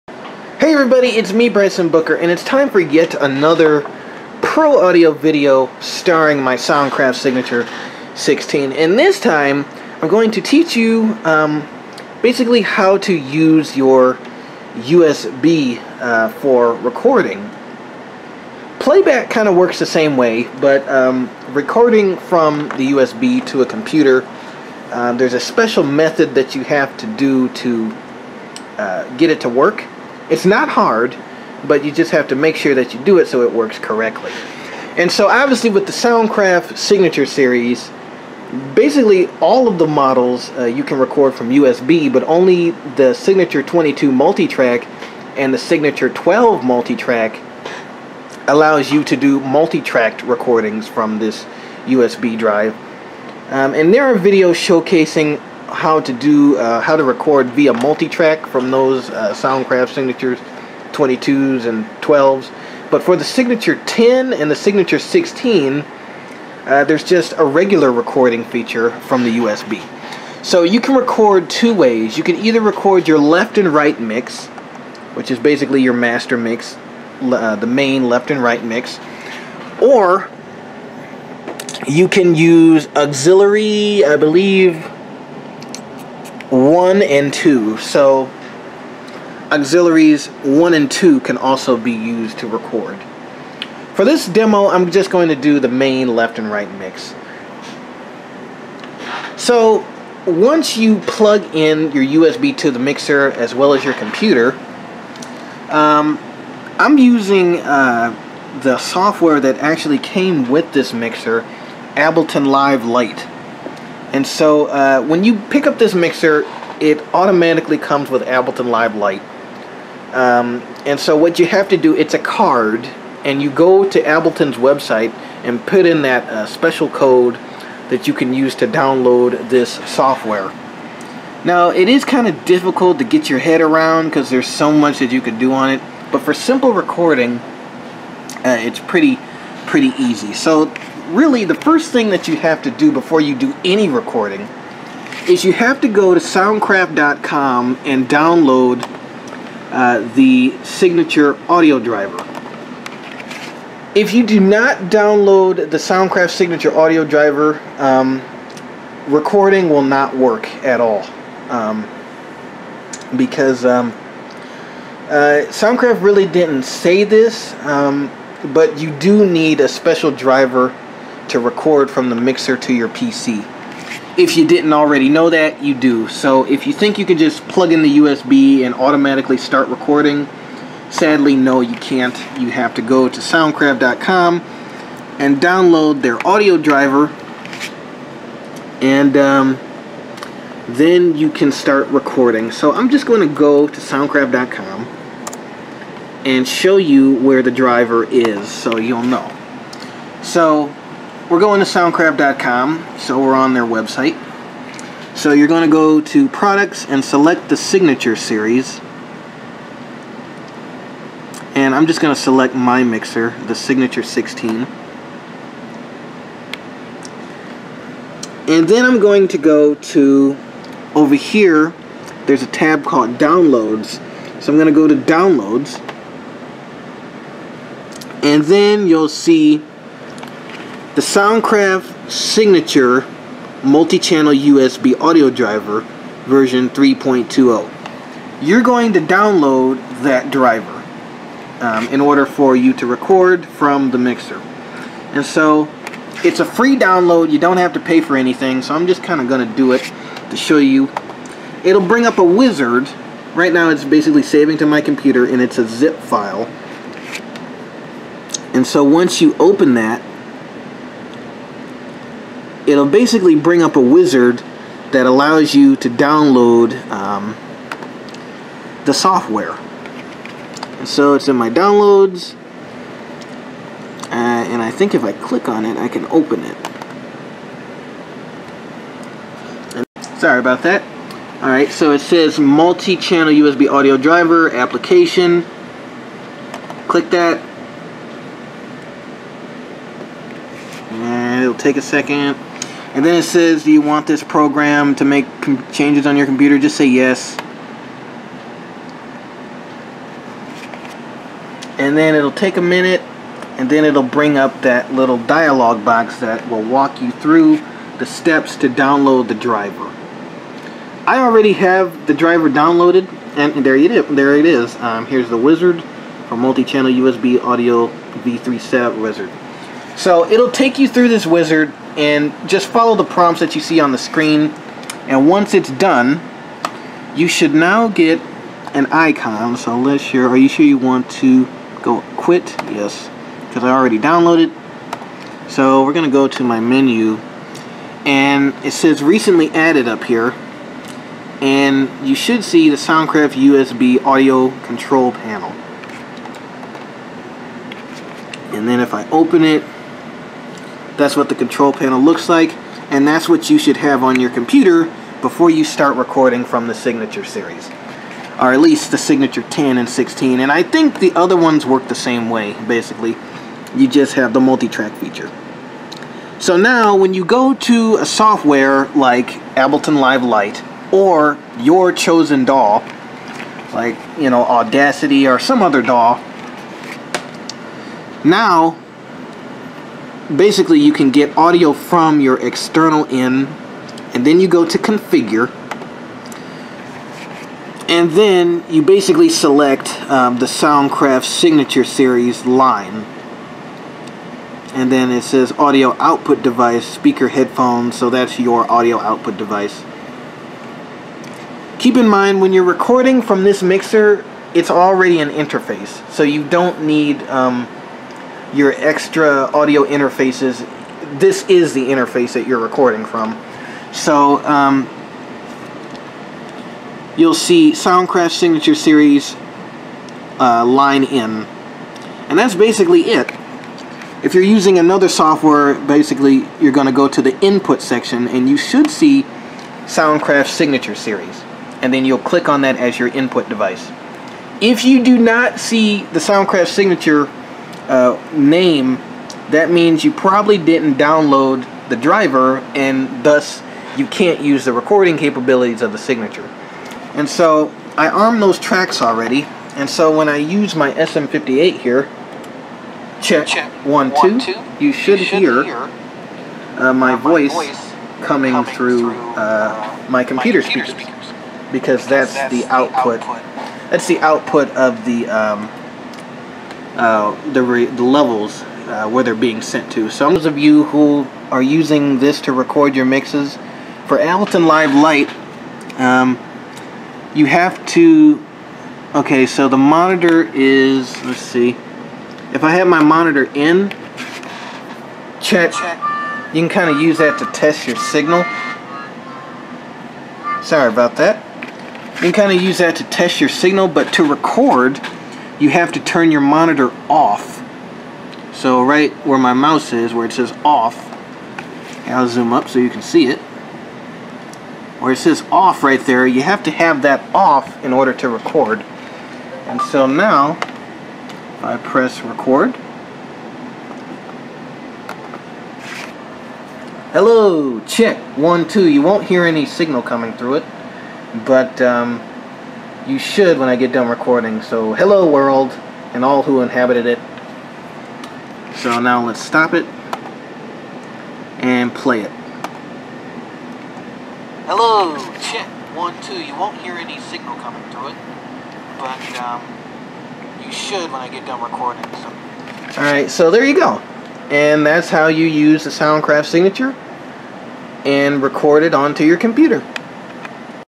How to record audio from your Soundcraft Signature (non multi track)
Today I give you a simple tutorial on how to send audio signals from your soundcraft signature via usb to a standard computer DAW. This tutorial is designed for all non multi track signature mixers.